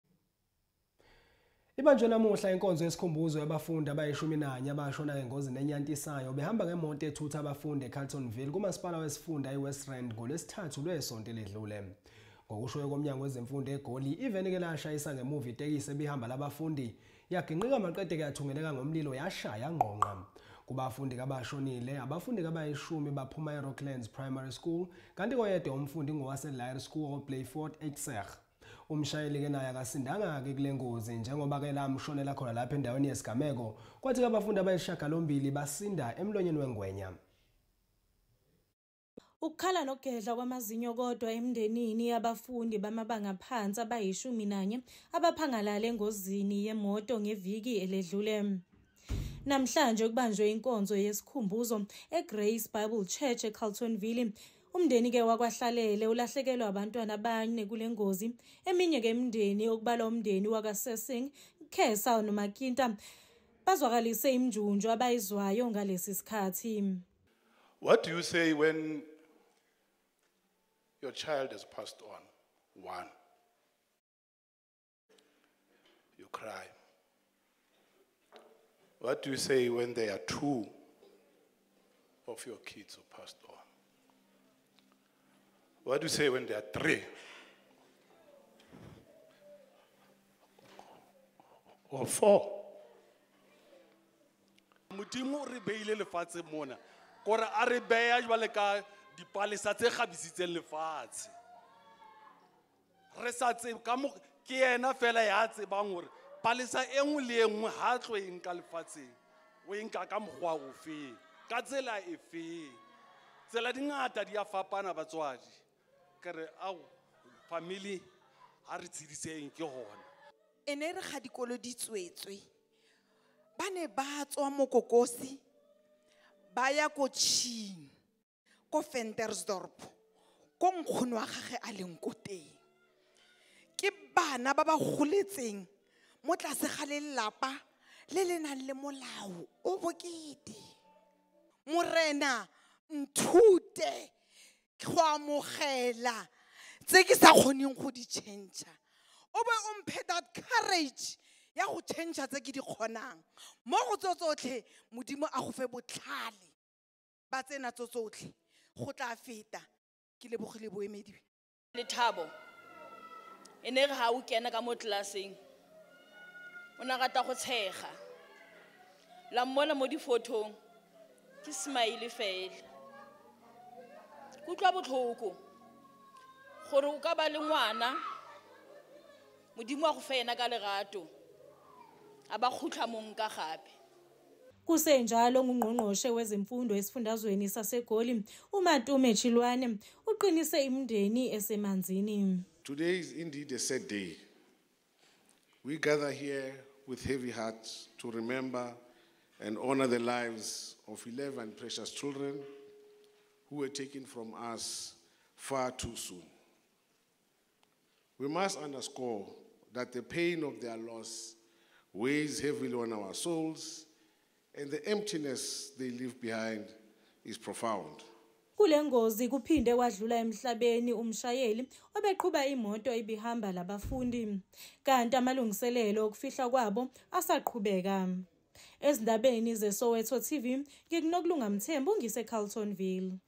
SOWETO TV NEWS | KUBANJWA INKONZO YESIKHUMBUZO ABASHONA ENGOZINI YEMOTO E-CARLETONVILLE
Ibanjwe namuhla inkonzo yesikhumbuzo yabafundi abayishumi nanye abashona engozini enyantisayo behamba ngemoto ethutha abafundi eCarletonville ngoLwesithathu ngesonto eledlule.